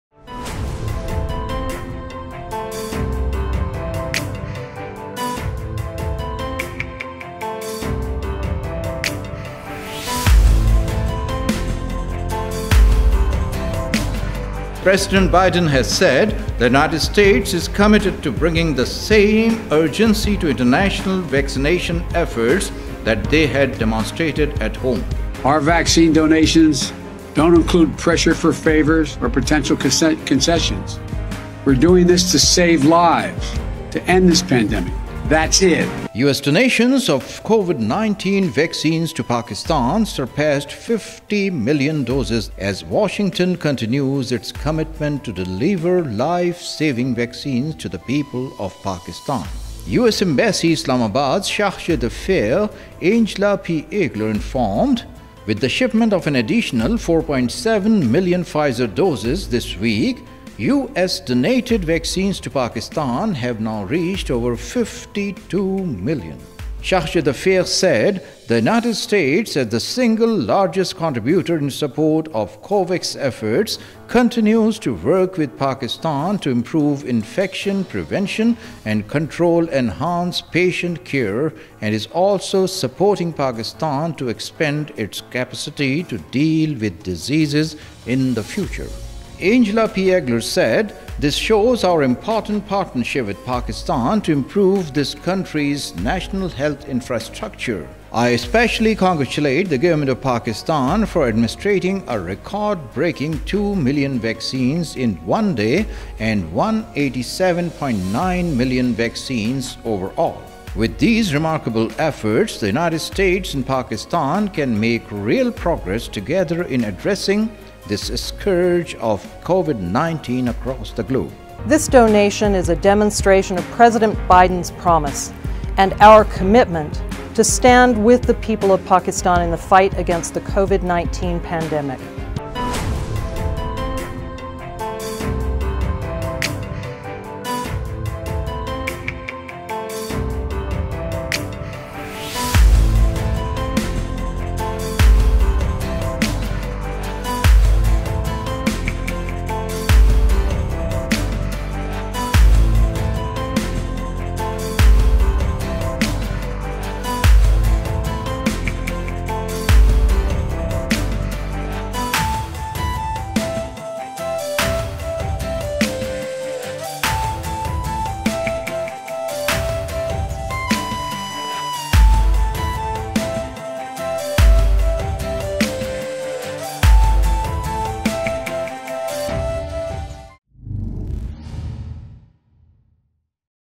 US donates 50M COVID-19 Vaccines to Pakistan| News Pakistan TV
M. M. ALAM
ISLAMABAD: US donations of COVID-19 vaccines to Pakistan surpassed 50 million doses this week, as Washington continues its commitment to deliver life-saving vaccines to the people of Pakistan.

US Embassy Islamabad’s Chargé d’affaires Angela P. Aggeler informed: “With the shipment of an additional 4.7 million Pfizer vaccine doses this week, U.S.-donated vaccines to Pakistan have now reached over 52 million.”

The United States delivered the most recent tranche of 4.7 million Pfizer vaccine doses to Pakistan late last week, bringing the total to over 52 million
U.S.-donated COVID-19 vaccine doses to Pakistan.

Chargé Aggeler said the United States, as the single largest contributor in support of COVAX efforts, continues to work with Pakistan to improve infection
prevention and control and enhance patient care and is also supporting Pakistan to expand its capacity to deal with diseases in the future. “This shows our
important partnership with Pakistan to improve this country’s national health infrastructure,” she said. “I especially congratulate the Government of
Pakistan for administering a record-breaking 2 million vaccines in one day and 187.9 million vaccines overall,” she added. “With these remarkable efforts,
the United States and Pakistan can make real progress together in addressing the scourge of COVID-19 across the globe.”

In addition to Pfizer and Moderna vaccines, the United States has also provided Pakistan with more than 1.2 million N95 masks, 96,000 surgical masks, 52,000
protective goggles, one million COVID-19 rapid diagnostic tests, 1,200 pulse oximeters, and 200 ventilators for 64 Pakistani hospitals. The U.S. government
also trained over 30,000 female health workers across Pakistan on home-based care for COVID-19 patients and established a national network of disease
surveillance and response units and teams. This is all part of the nearly $70 million the U.S. government has donated to assist the Pakistani people in the
fight against COVID-19.